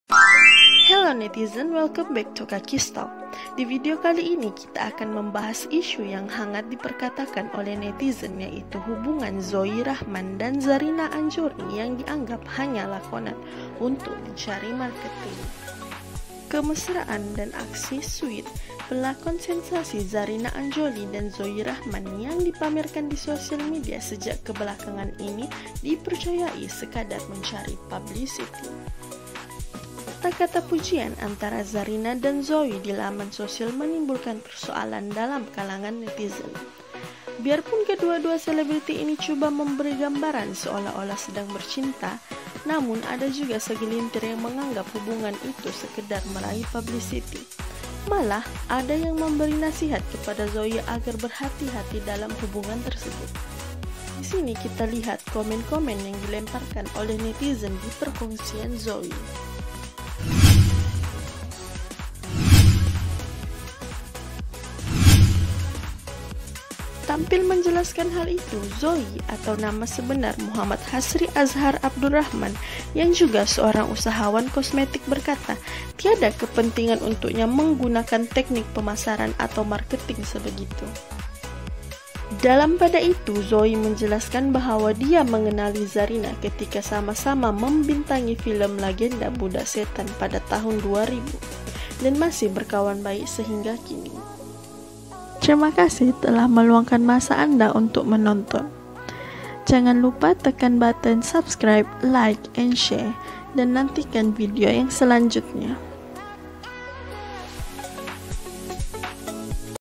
ZOEY RAHMAN DAN ZARINA ANJOULIE BUAT DRAMA MARKETING ??BERLAKON "SWEET" SAHAJA.
" NETIZEN HIDU ZOEY RAHMAN DAN ZARINA ANJOULIE BUAT DRAMA MARKETING, BERLAKON "SWEET" SAHAJA " - JANGAN RAPAT NANTI BELI BOXER PUN DIUNGKIT!! " artismalaysia artist kontroversi hiburan # gossip